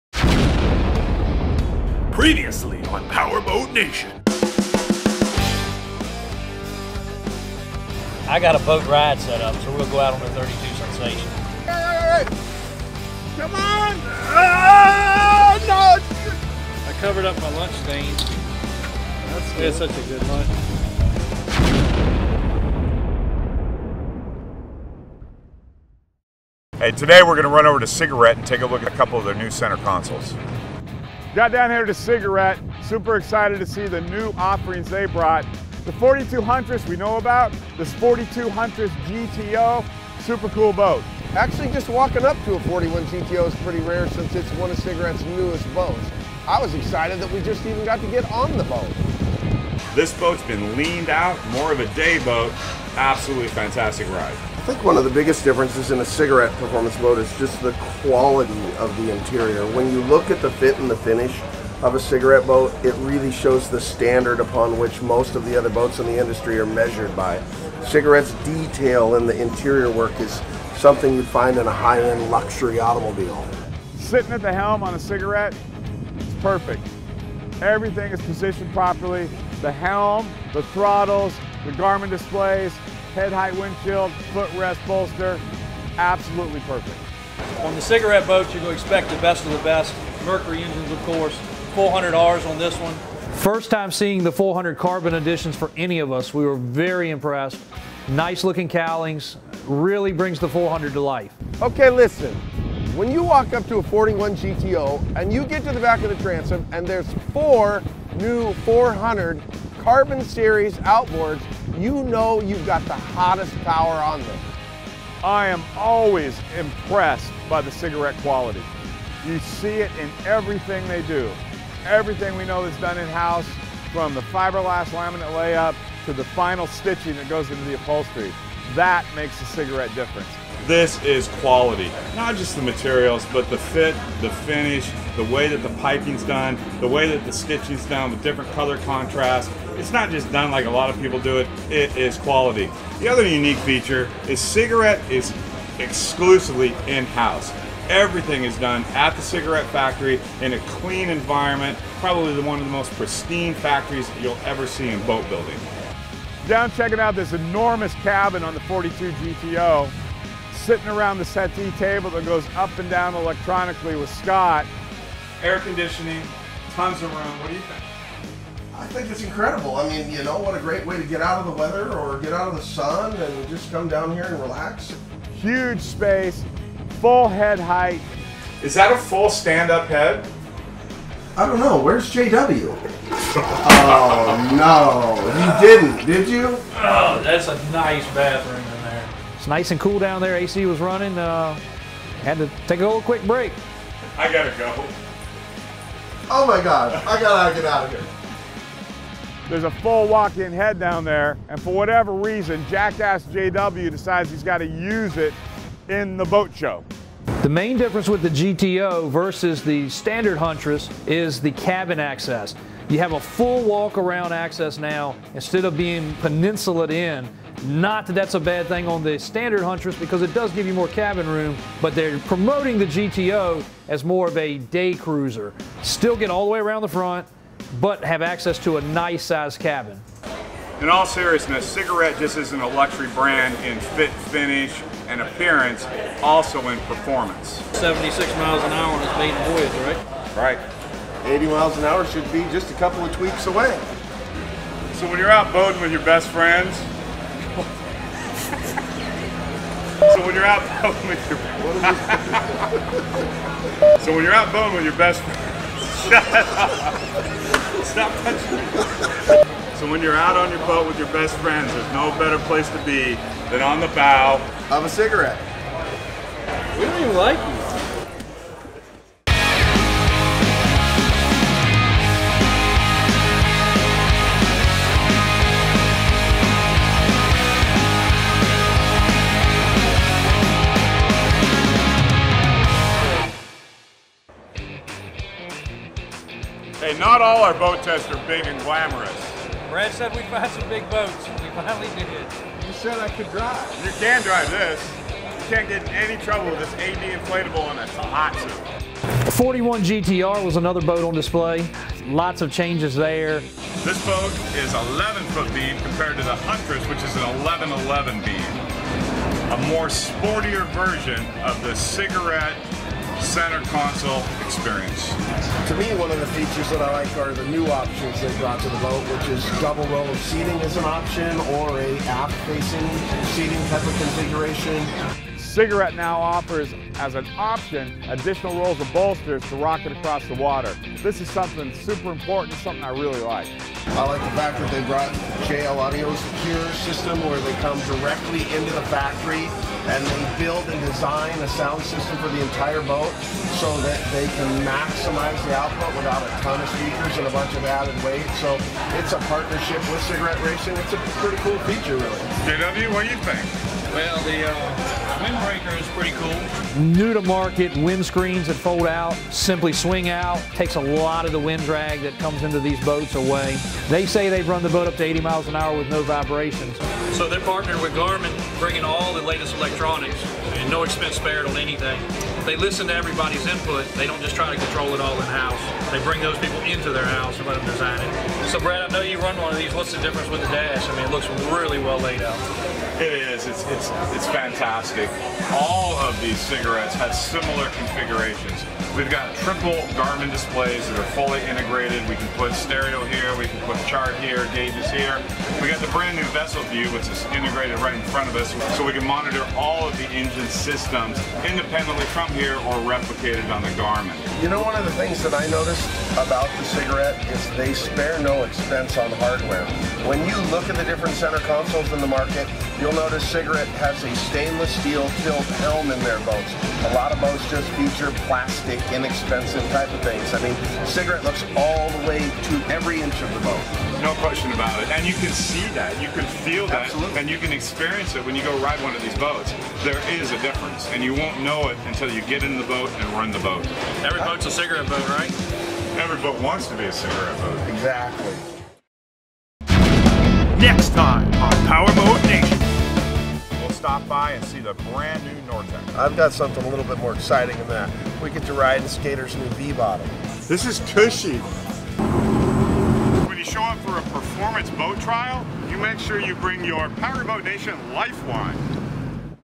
Powerboat Nation Rocks the 2017 Miami Boat Show - Part Four
In this episode, we’re in for round four at the 2017 Miami Boat Show. After checking out the AMG–Cigarette 50 Marauder, testing out the awesome 426 SKATER and 32 Sunsation CCX from Performance Boat Center, we’re taking a look at two center consoles from Cigarette, the 42 Huntress, and 42 Huntress GTO. These feature packed center consoles have plenty to love and the performance to match.

Thanks to Cigarette Racing for running footage of the 42 Huntress GTO.